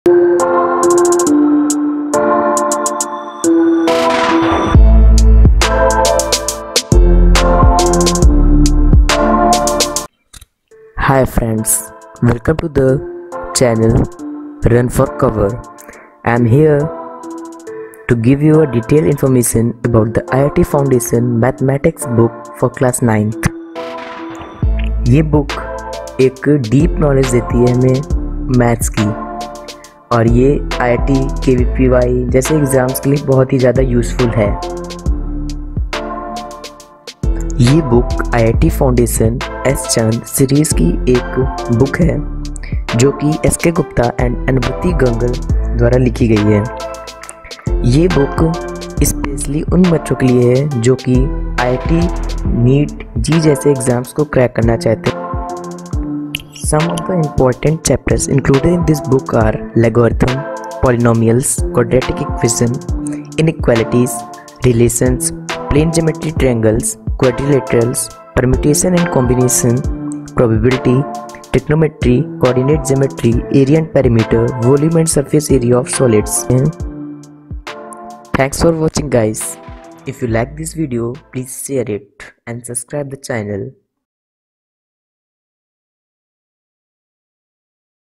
Best IIT Foundation Book Class 9 Mathematics || Run For Cover || S. Chand || S.K. Gupta
Hey,
this video is about the best book For IIT Foundation Mathematics Class 9.

IIT Foundation Mathematics
S.K. Gupta
s. chand
best book of class9